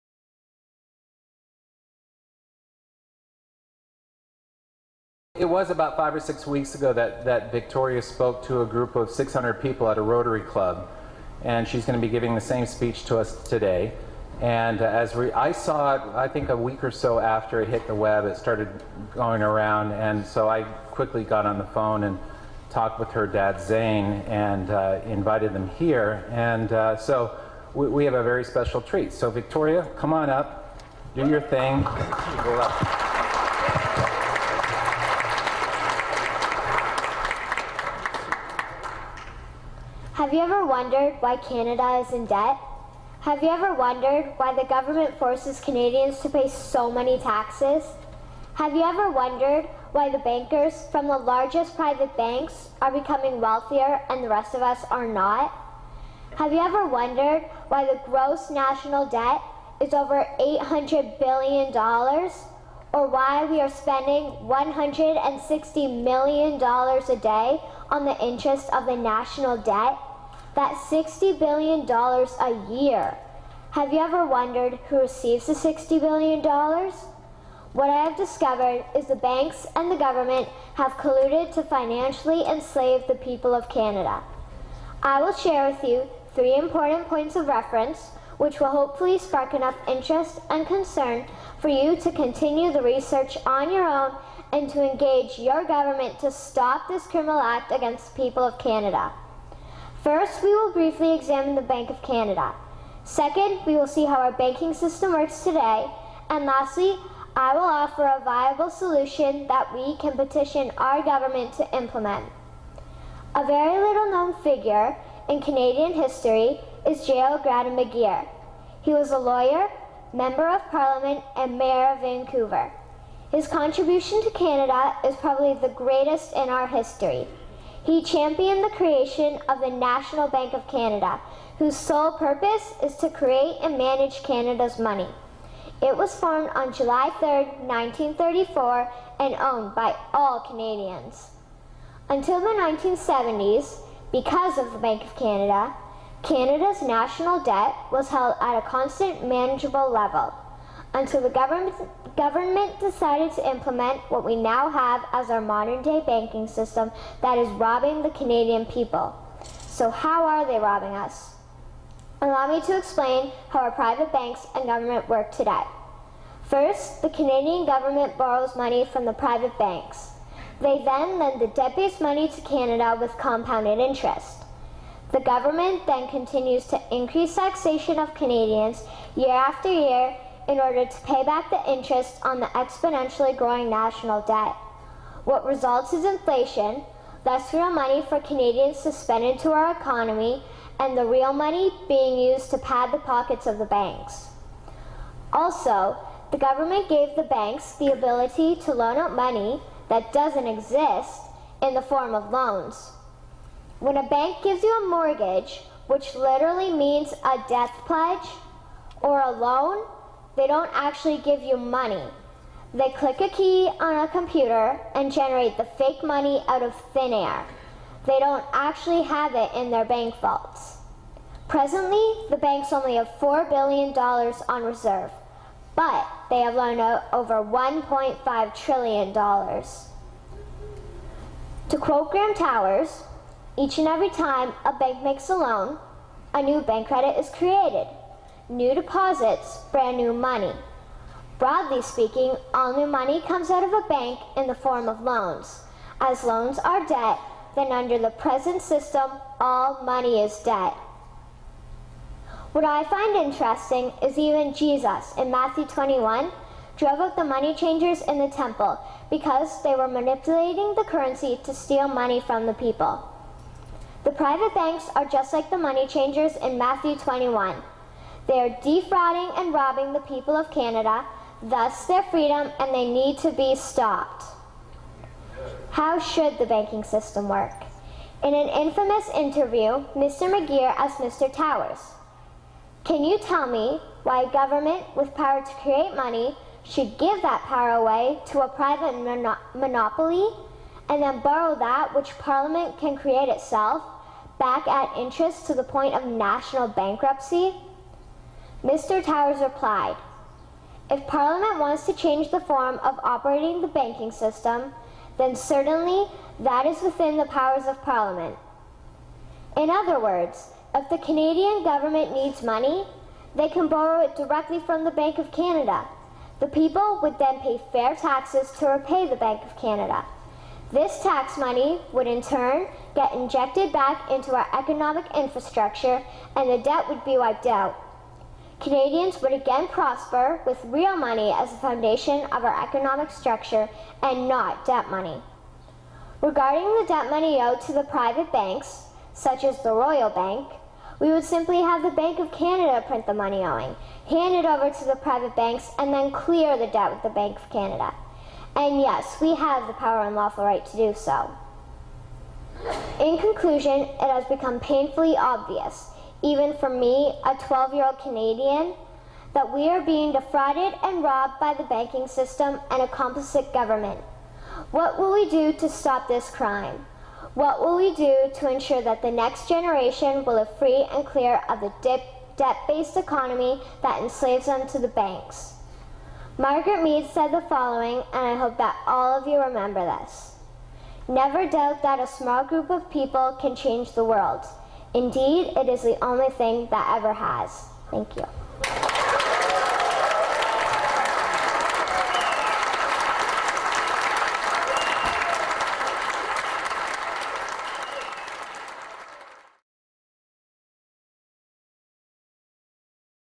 12 Year Old Child tells us about the corruption in the world banking system
Victoria is a 12 year old Canadian who tells all in laymans terms about the huge scandal and corruption involved in putting people into debt. The private bankers and the government act together to dupe and steal tax payers money.